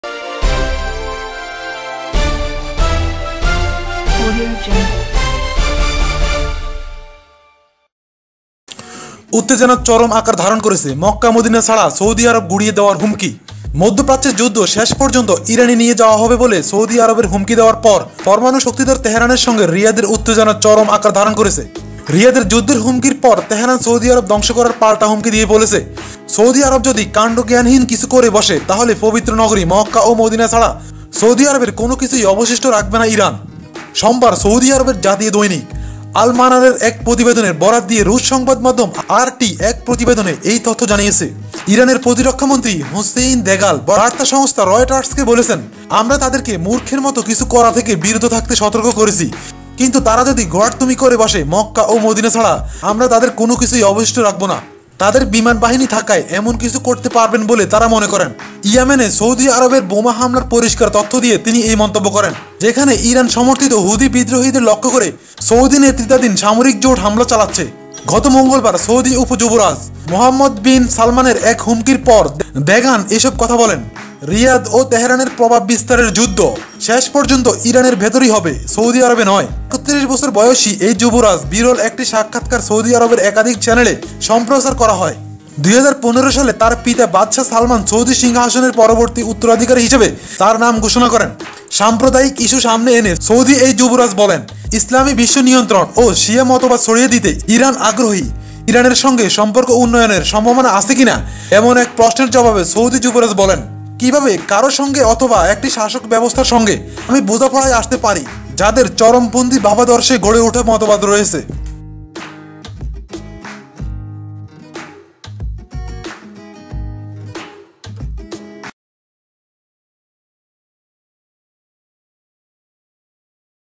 এবার ‘মক্কা মদিনা’ ছাড়া সৌদি আরব গুঁড়িয়ে দেয়ার হুমকি, দেখুন বিস্তারিত
এবার ‘মক্কা মদিনা’ ছাড়া সৌদি আরব গুঁড়িয়ে দেয়ার হুমকি